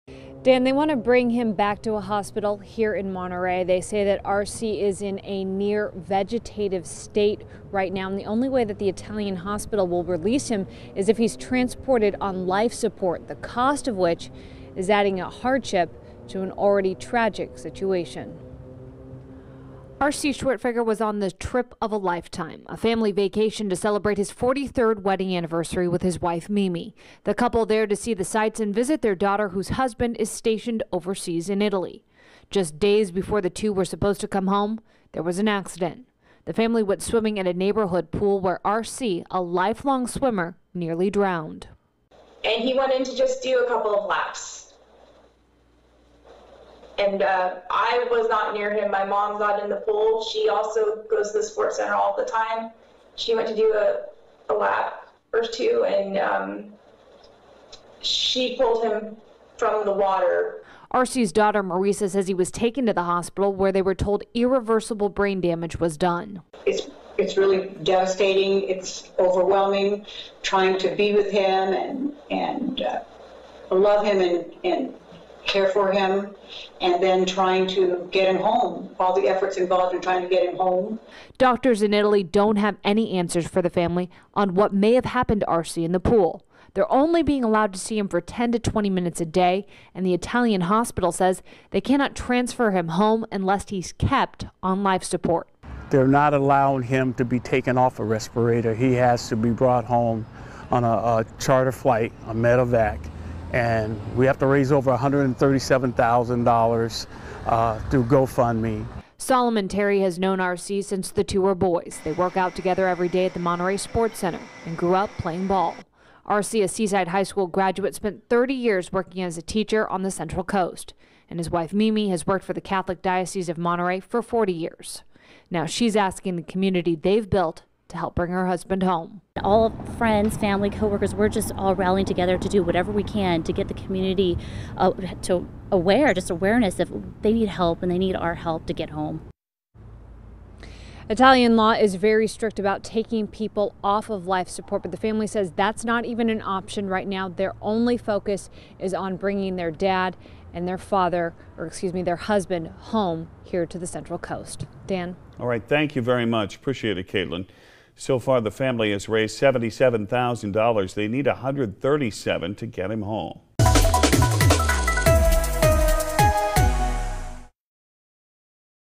Salinas man trapped in Italy after medical emergency
R.C. Schwertfeger nearly drowned while on vacation in Italy and suffered massive brain damage. His family is attempting to raise over $130,000 for a medivac flight to get him back home.